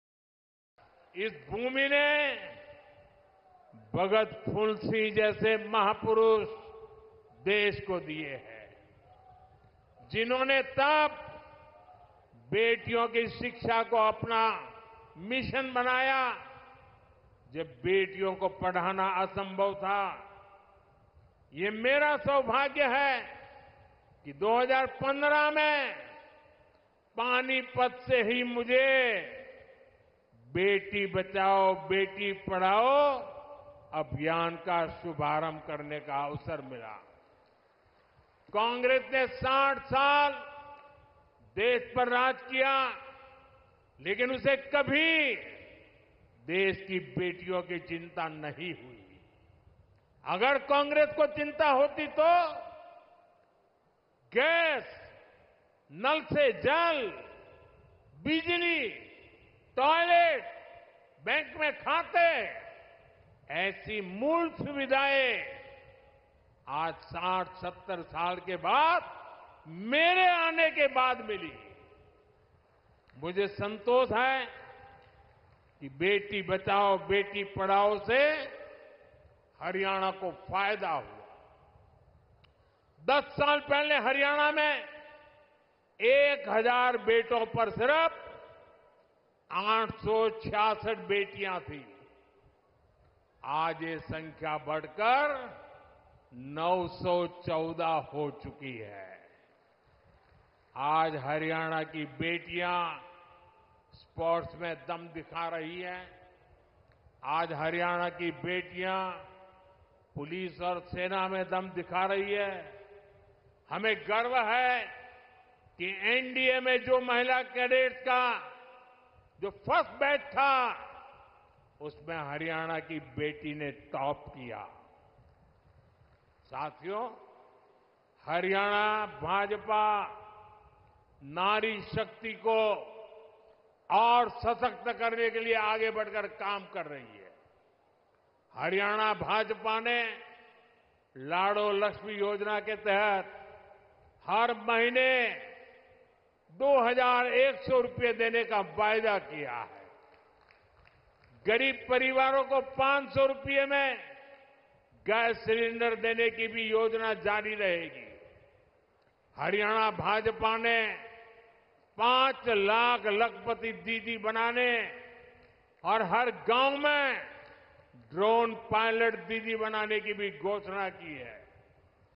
आज हरियाणा की बेटियां पुलिस व सेना में दम दिखा रही है
हर क्षेत्र में अपना दमखम दिखाने वाली हरियाणा की नारीशक्ति को और सशक्त करने के लिए हम कोई कोर-कसर नहीं छोड़ रहे हैं।